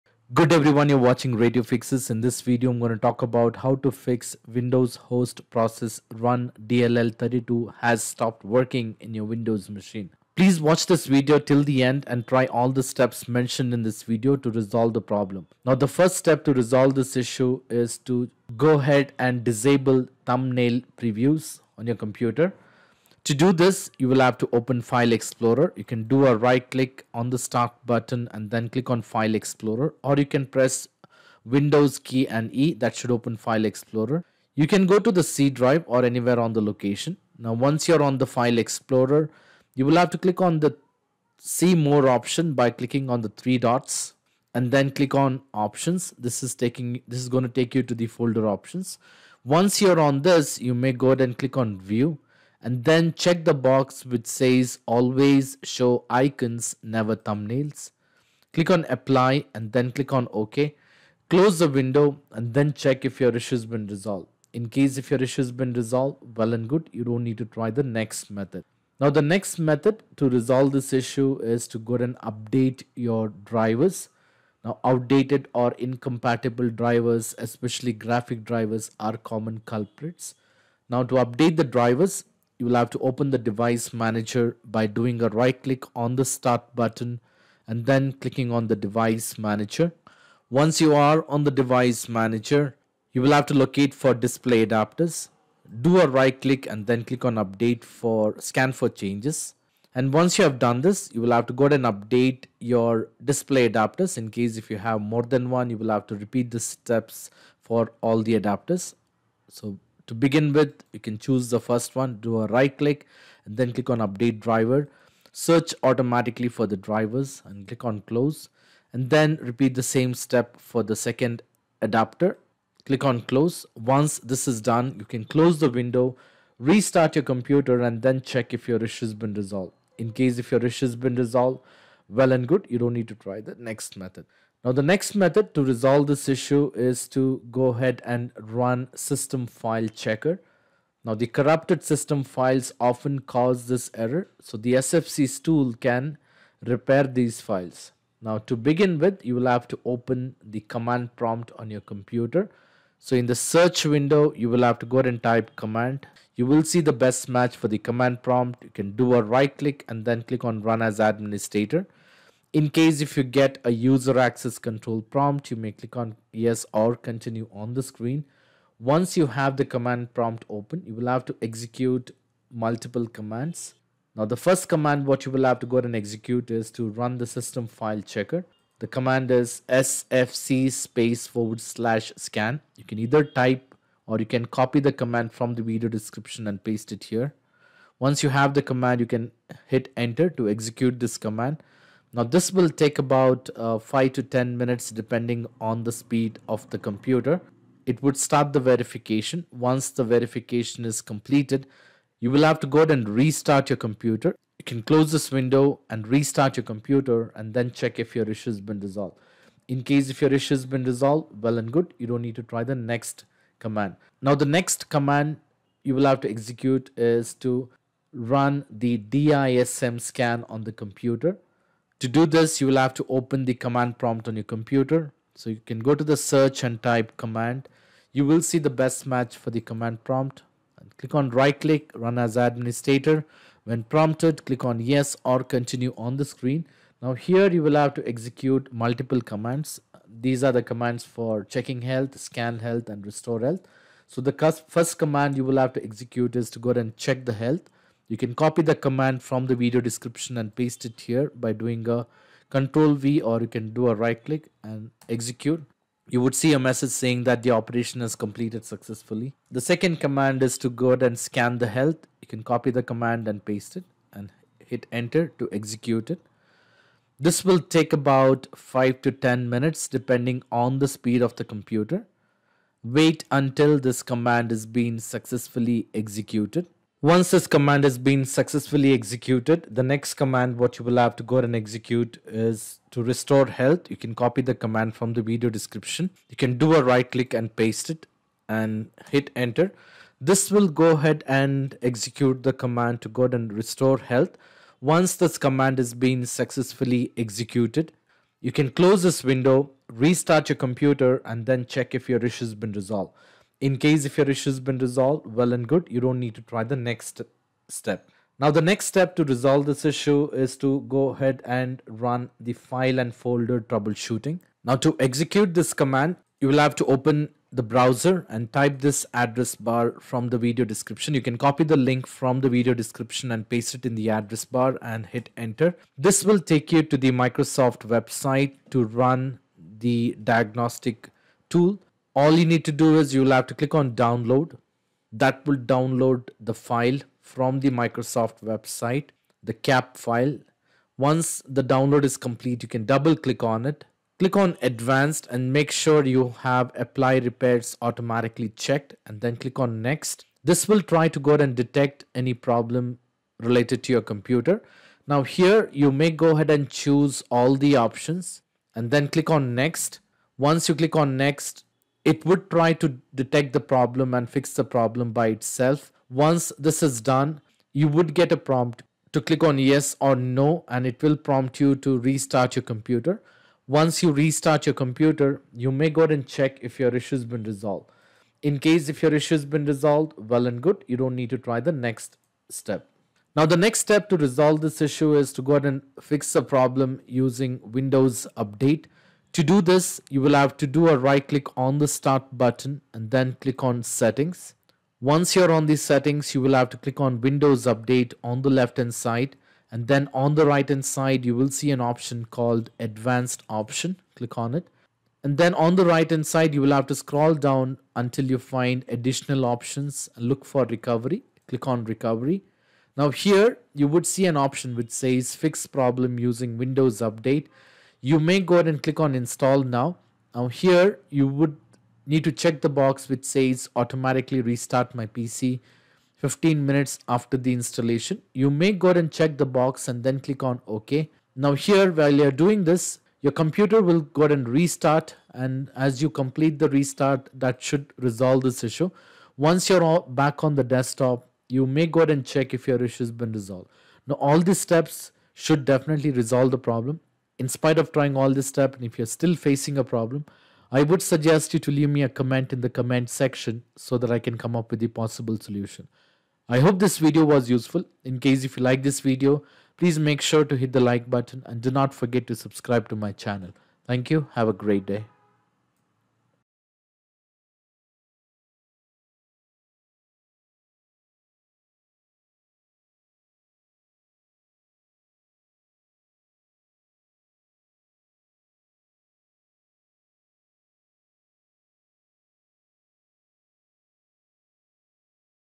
Solved: Windows Host Process RunDLL32 Has Stopped Working in Windows 11 /10
Fixing "Windows Host Process RunDLL32 Has Stopped Working" in Windows 11 and Windows 10 :

The "RunDLL32 has stopped working" error in Windows 11 typically appears due to corrupted system files, outdated or faulty drivers, malware infections, or software conflicts. Below are effective solutions you can follow to resolve this issue:

1 : Disable Thumbnail Previews :
This fix is helpful if the error appears when using File Explorer:
Open File Explorer and select the View tab
Click Options -- the View tab
Check Always show icons, never thumbnails -- Click Apply -- OK

2. Update Drivers :
Outdated or incompatible drivers, especially graphics drivers, are common culprits:
Right-click Start -- Device Manager
Expand Display Adapters
Right-click your graphics card -- Update driver
Choose Search automatically for drivers
Also, check Windows Update for pending updates and install them

3. Run System File Checker (SFC):
Corrupted system files often cause this error. The SFC tool can repair these files:
Press Windows + S, type cmd, and right-click to Run as administrator
Type: sfc /scannow
Press Enter and wait for the scan to finish
Restart your PC after completion

4.Run DISM Scan
If SFC doesn't fix the issue, use DISM to repair the Windows image:
pen Command Prompt as Administrator
Enter these commands one by one:
Let the process complete before restarting your computer.

6. Fix the Problem using Windows Update